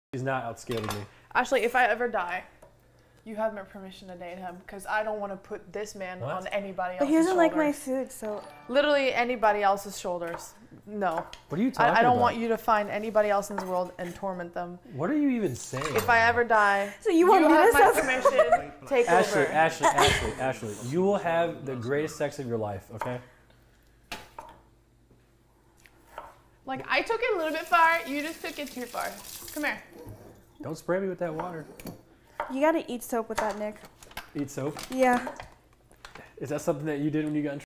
Malena gives Nick permission to date someone else
-I'll be uploading daily best Twitch clips
-I hope you guys enjoy this Twitch clip and hope to bring you more Twitch clips in the future!